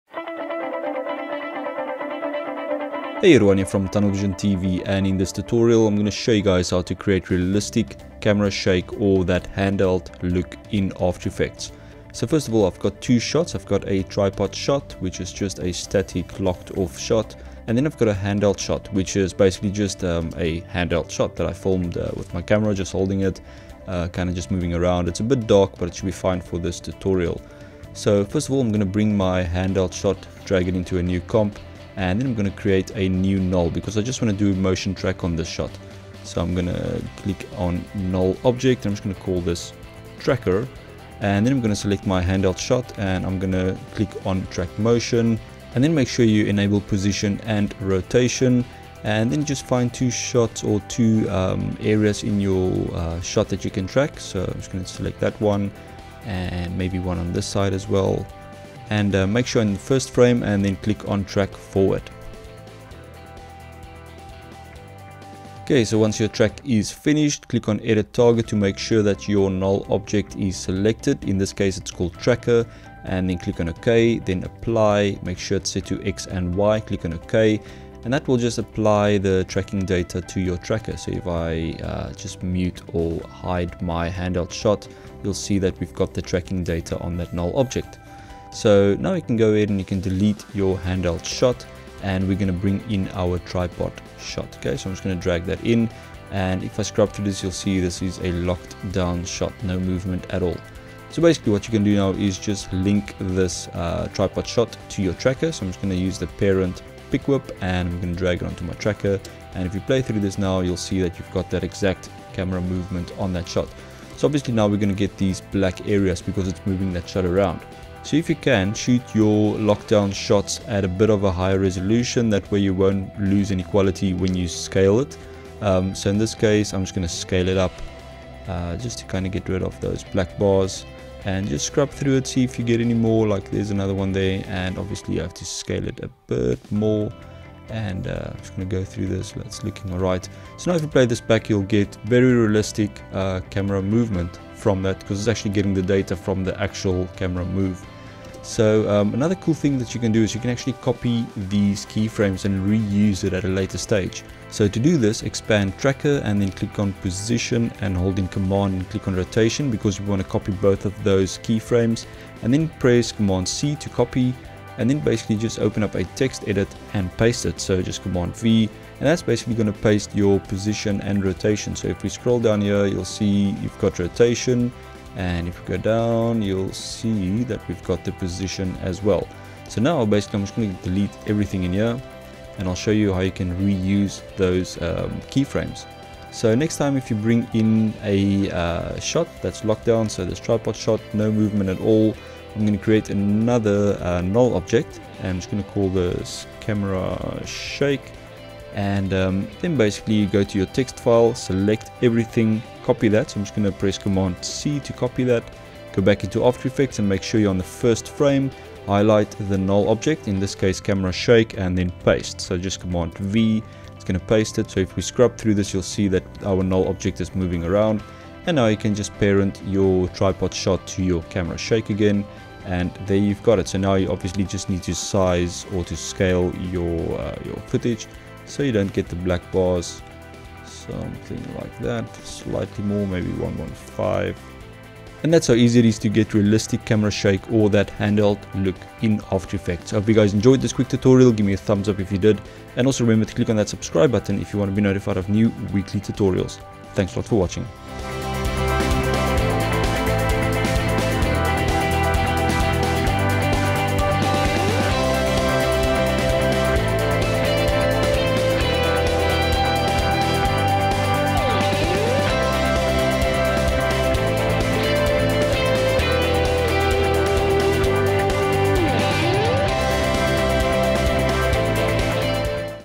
After Effects Tutorial - How To Create Realistic Camera Shake
After Effects Tutorial - How To Create Realistic Camera Shake In After Effects - VFX Tutorial

In this After Effects tutorial I show you how to create realistic camera shake by sampling real handheld footage.

Ruan
TunnelvizionTV